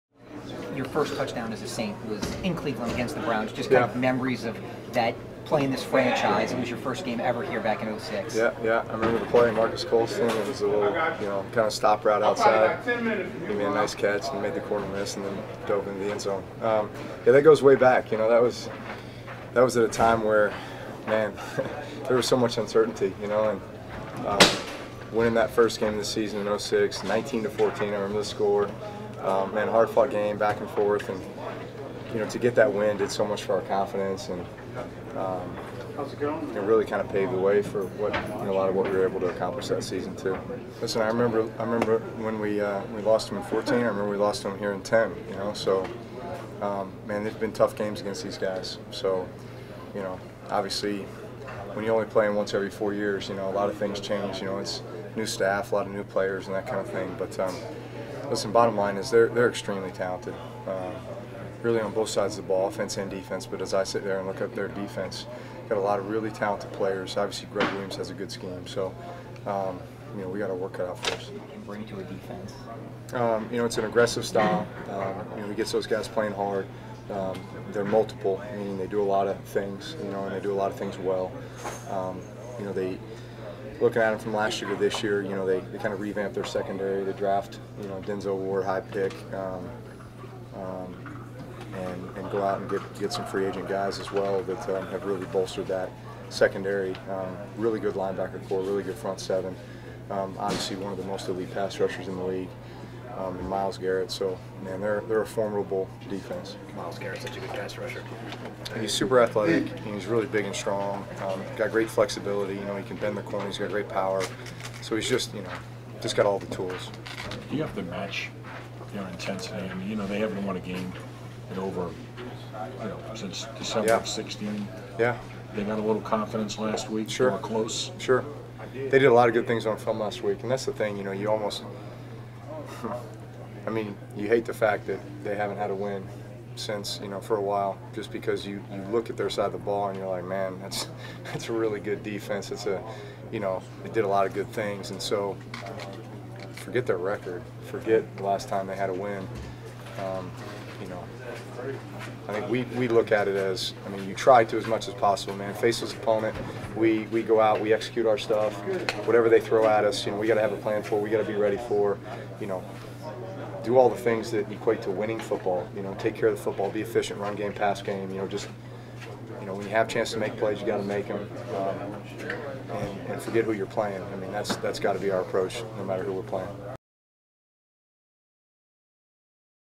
See what Drew Brees has to say about Cleveland Browns, pass rusher Myles Garrett
New Orleans Saints quarterback Drew Brees spoke to the media on Wednesday about facing the Cleveland Browns and elite pass rusher Myles Garrett in Week 2.